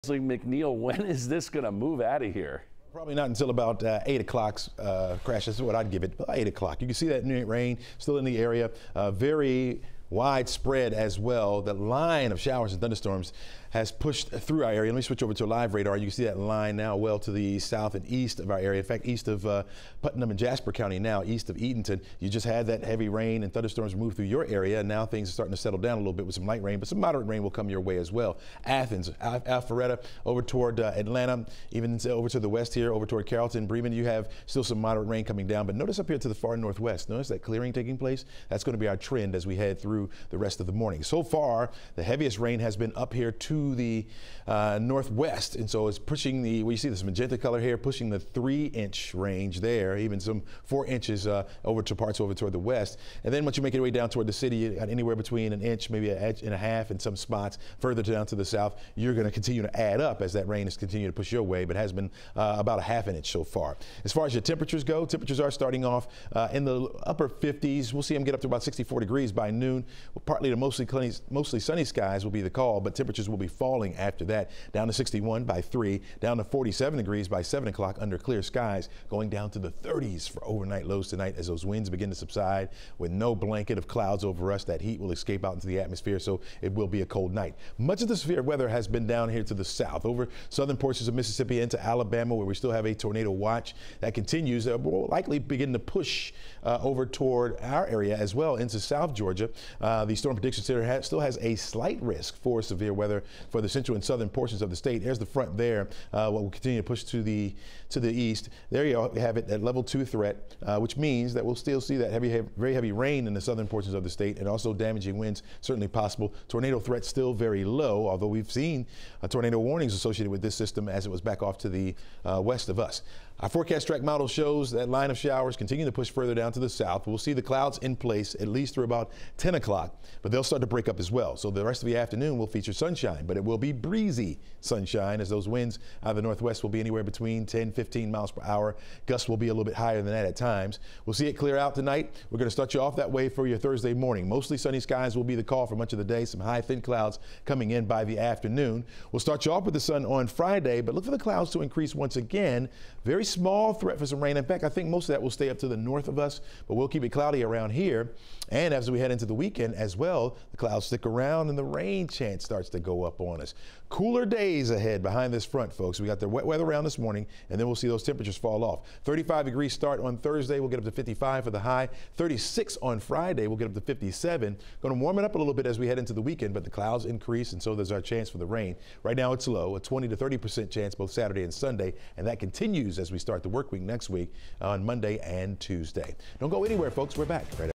When heavy rain will move out of metro Atlanta
Heavy rain has left flooded roads and downed trees in metro Atlanta.